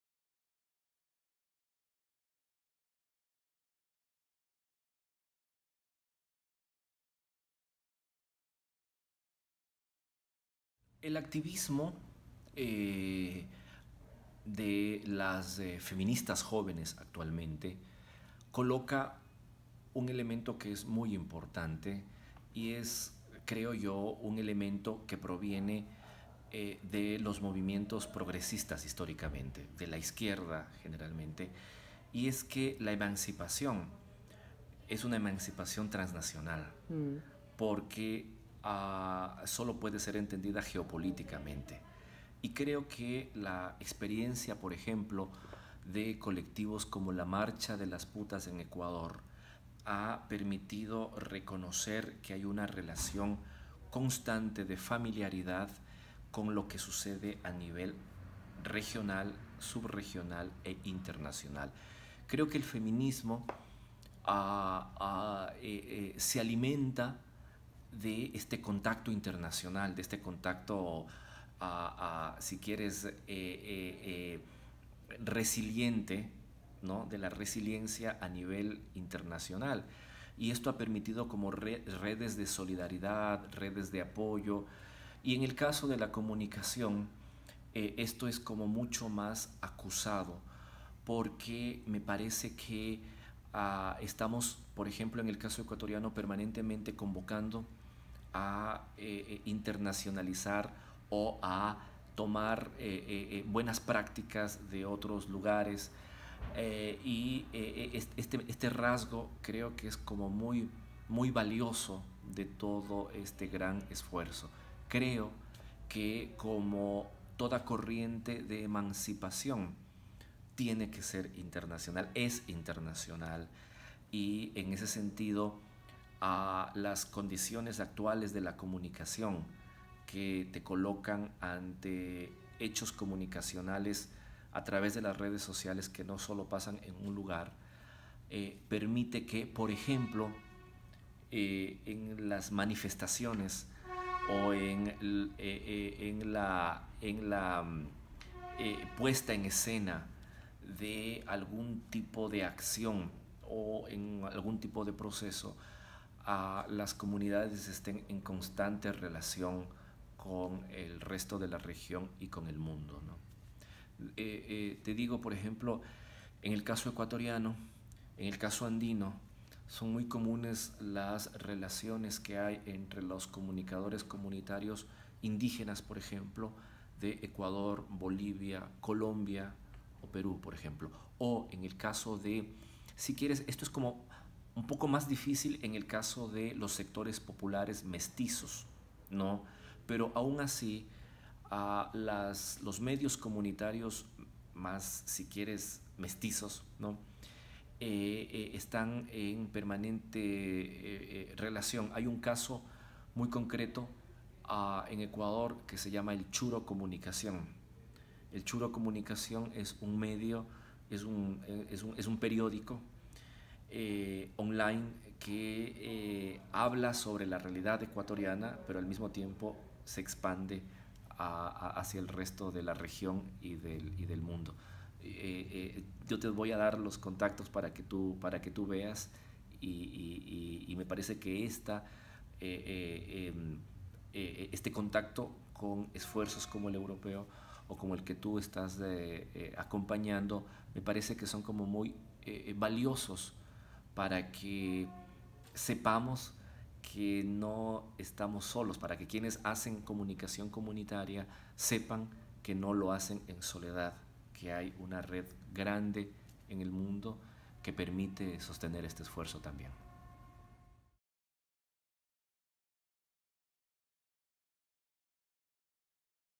EDGAR VEGA SURIAGA, Universidad Andina Simon Bolivar, transnational advocacy
Edgar Vega Suriaga, from the Universidad Andina Simon Bolivar in Ecuador, talks about local and national advocacy initiatives to promote gender equality in the media.